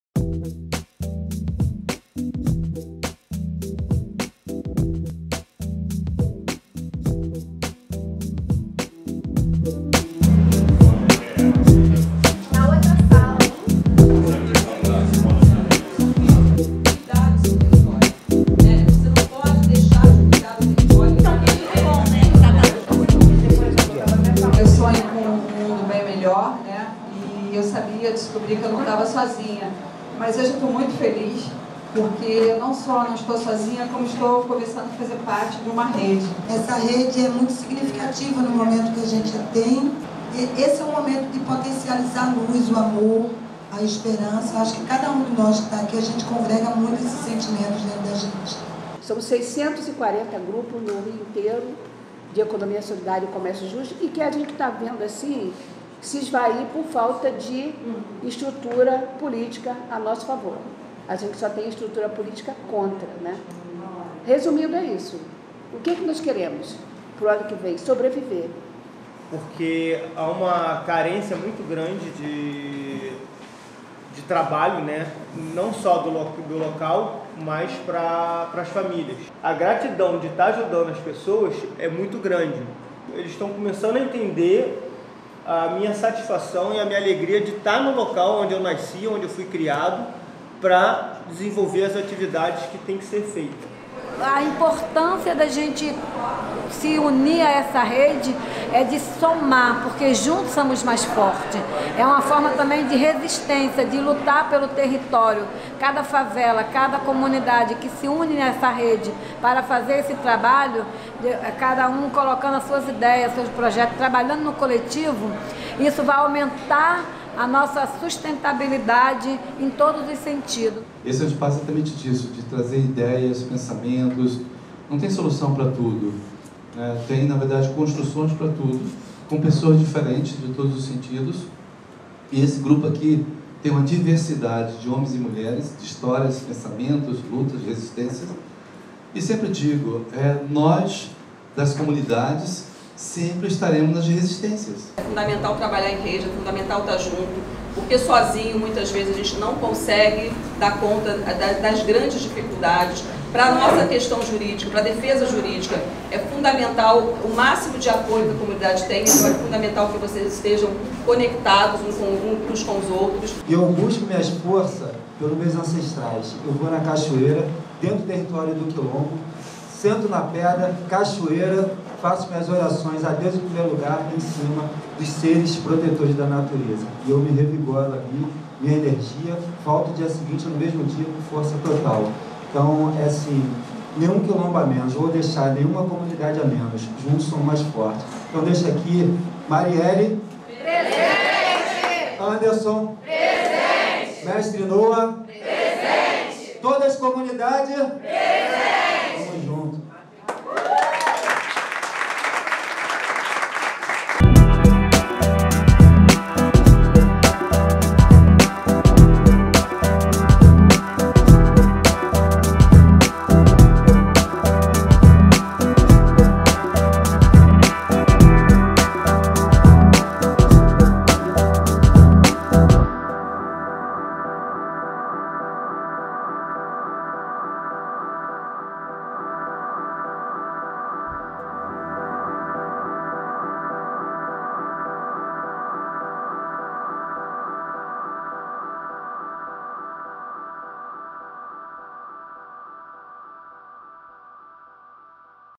1o Grande Encontro da Rede Favela Sustentável 2018 | 1st Sustainable Favela Network Meet-Up 2018
“Cada um de nós tem uma missão… Juntos, somos mais fortes”, declarou Adilson Almeida, fundador da Associação Cultural Quilombo do Camorim (ACUQCA)—uma das agora mais de 120 iniciativas de sustentabilidade e resiliência mapeadas pela Rede Favela Sustentável (RFS) no Rio de Janeiro. “Temos sempre que resistir, lutar e nunca desistir.”

Adilson compartilhou esses pensamentos com mais de 130 mobilizadores comunitários e aliados, congregando-se pela primeira vez para o lançamento formal da RFS. A reunião, ocorrida no dia 10 de novembro de 2018 na Fundação Escola da Defensoria Pública do Estado do Rio de Janeiro (FESUDEPERJ), foi um dia tanto de trabalho como de festividade—os participantes se agregaram não apenas para trocar ideias, estratégias e experiências, mas também para celebrar a força na solidariedade e a determinação dos moradores de favela ao enfrentar desafios que estão diante de suas comunidades.

Da Baixada Fluminense à Cidade de Deus, participantes vieram de toda a Região Metropolitana do Rio de Janeiro para participar do lançamento do evento. Mais da metade dos presentes era de mulheres. Moradores da favela, que compunham uma maioria, se juntaram a pesquisadores e aliados técnicos que trabalham com iniciativas focadas em sustentabilidade tanto dentro como fora das favelas.

Vídeo por Ian de Farias.